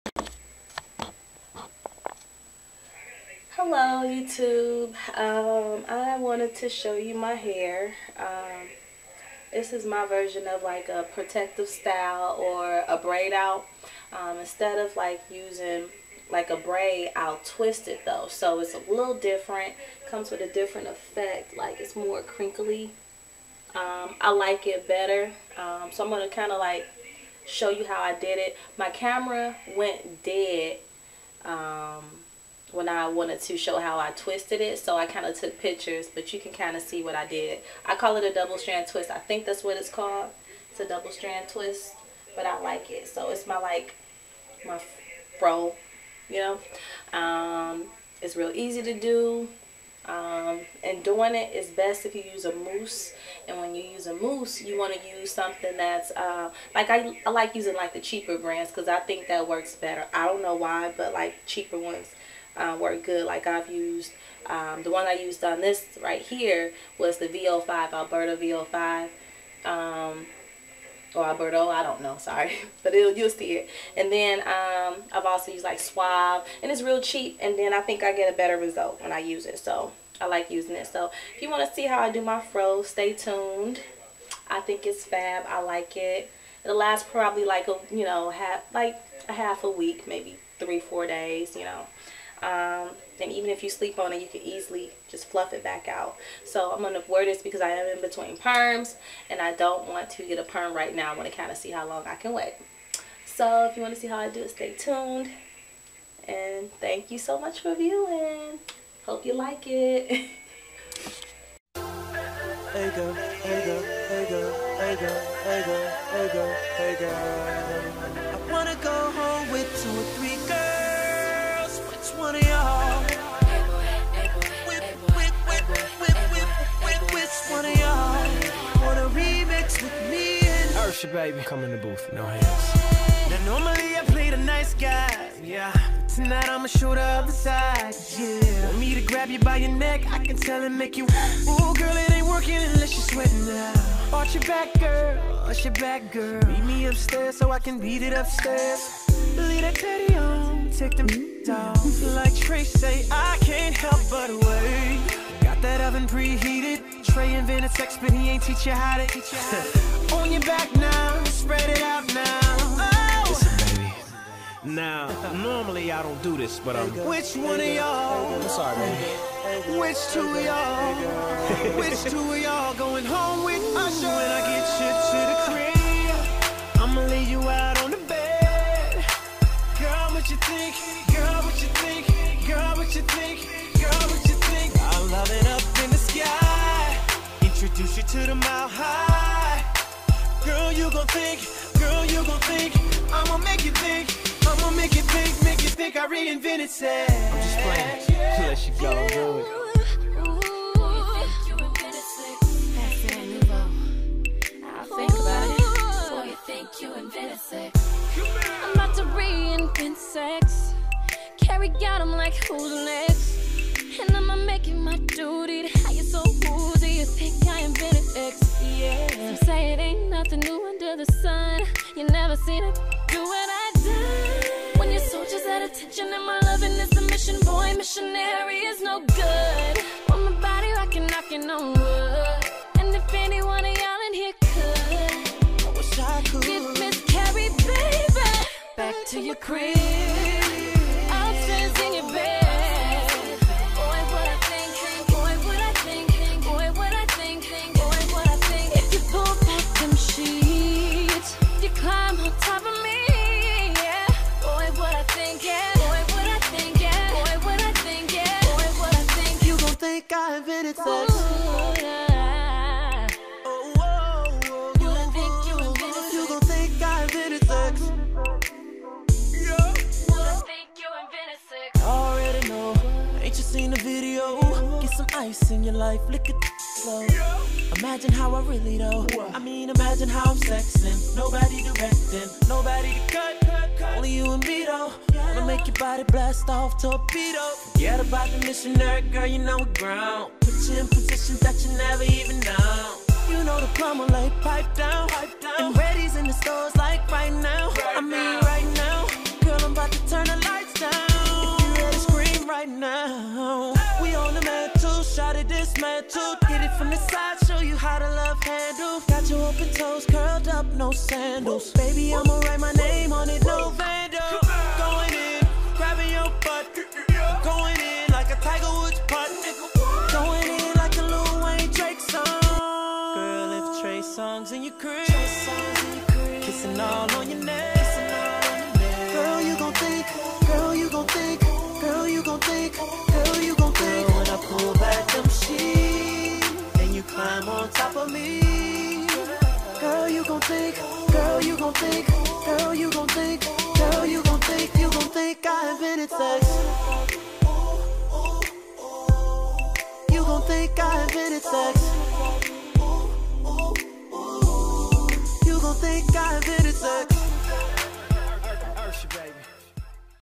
Double Strand 'Twist Out' Protective Curly Look
This is how I do my 'twist out'! The braid out is good too, but I prefer this turnout.

Feel free to LIKE or COMMENT! Both are welcome!